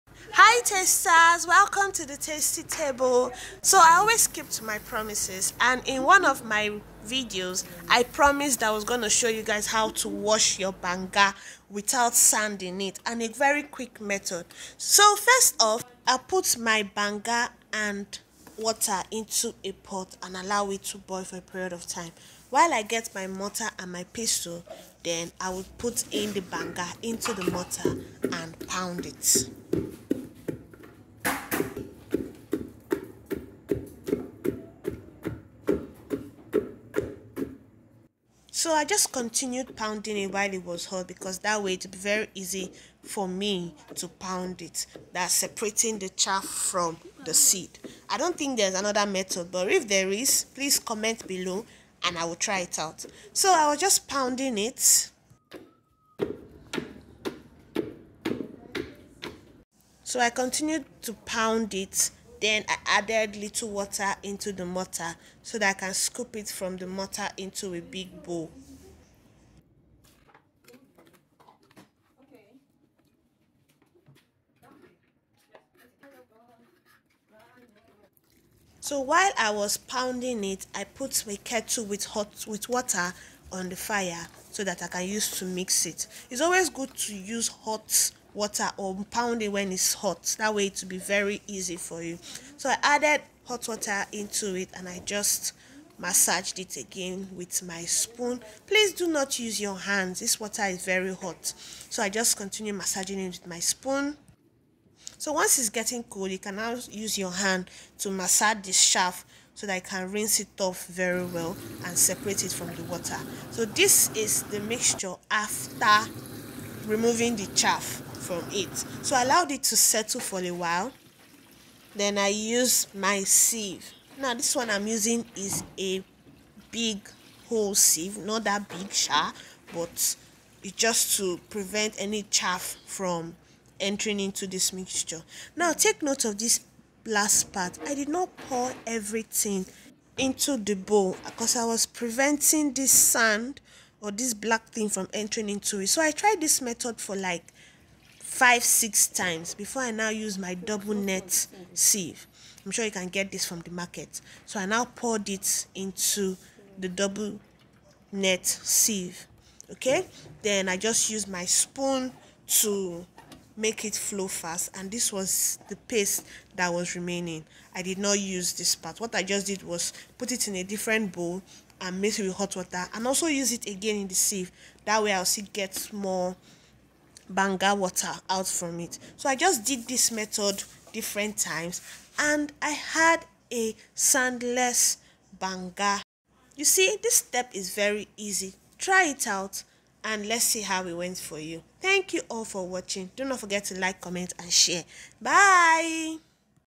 How to Extract Palmoil with your Hands||Banga||Very easy process
This process is very easy, and you will have NO SAND at the end